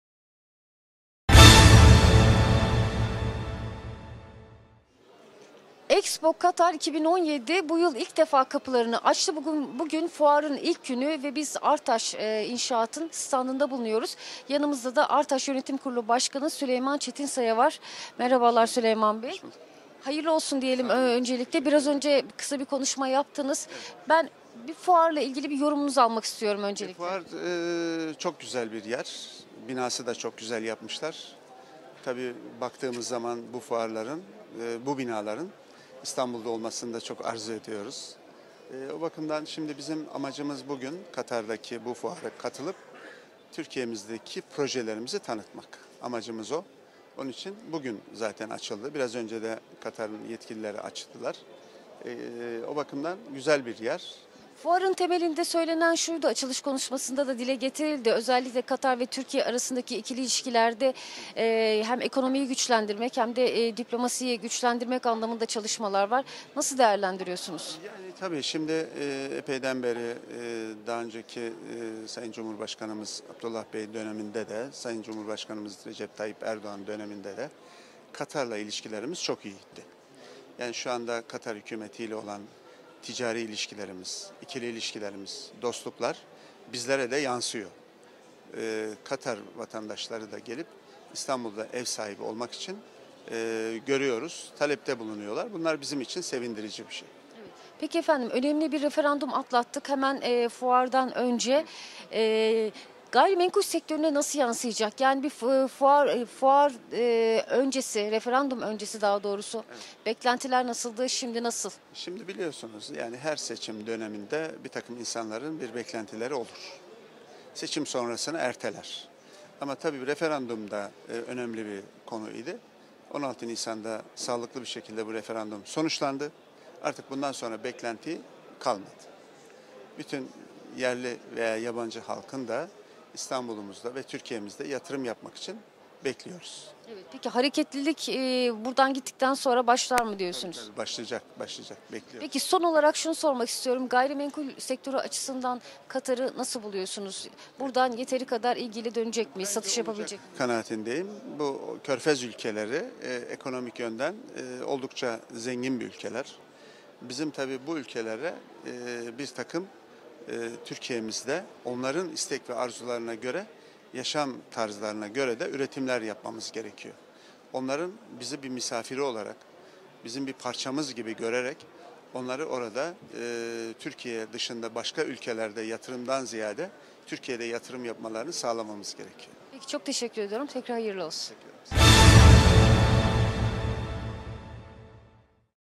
Süleyman Çetinsaya Expo Turkey by Qatar fuarını değerlendirdi
Artaş Holding Yönetim Kurulu Başkanı Süleyman Çetinsaya bu sene ilk defa düzenlenen Expo Turkey by Qatar fuarı hakkında Hatice Kolçak'ın sorularını yanıtladı.